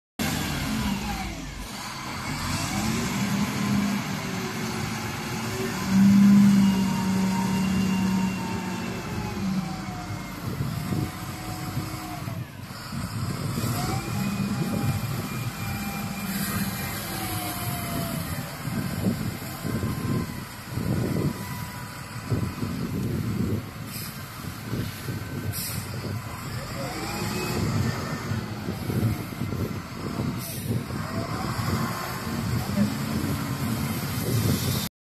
XCMG ZL50GN loader is loading materials for the dump truck
XCMG ZL50GN loader is loading materials for the dump truck. In the 2 years of using XCMG loader, XCMG loader has high attendance rate, low failure rate, low fuel consumption and good economy, and has won high recognition from customers.